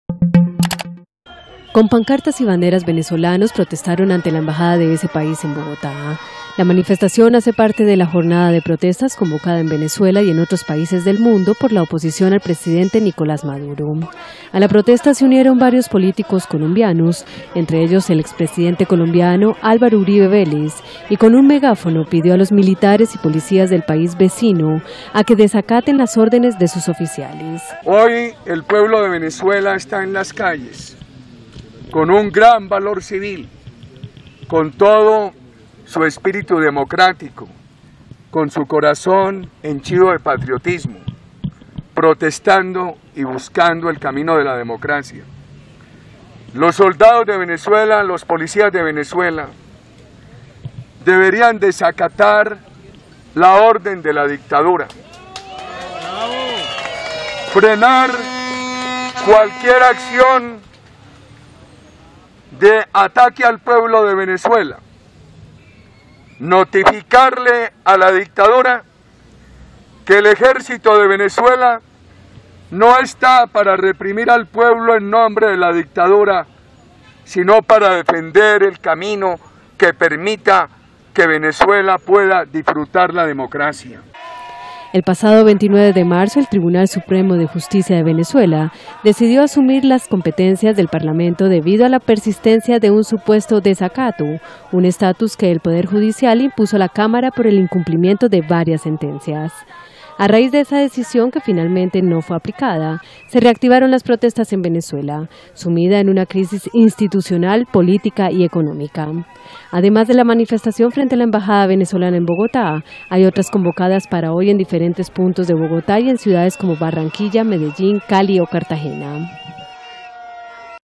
Venezolanos y políticos protestan ante embajada de ese país en Bogotá
Bogotá, 19 abr (EFE), (Imagen: Juan Pablo Alemán).- El expresidente colombiano Álvaro Uribe se sumó hoy a las protestas de varios centenares de venezolanos ante la embajada de ese país en Bogotá, desde donde exhortó a los militares y policías del país vecino a que "desacaten" las órdenes de sus oficiales.